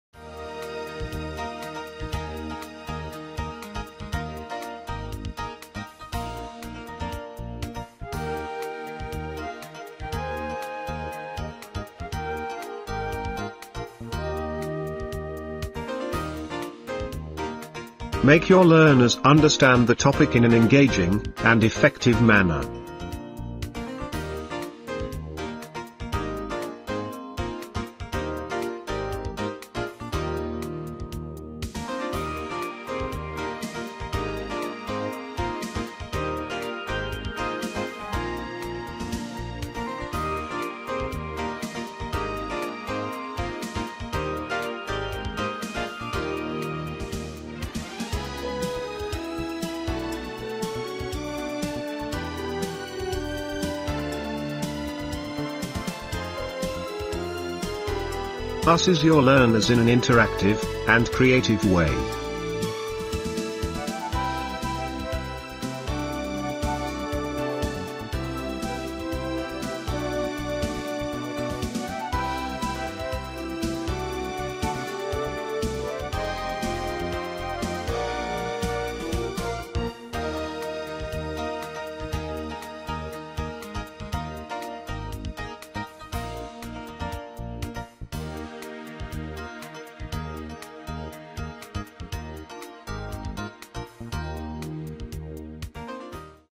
Interactive eLearning course on CFO's Key Competencies
This video is simplified learning on the key competencies of CFOs.
eLearning Goodies can create customized and engaging courses based on the requirements of your organization.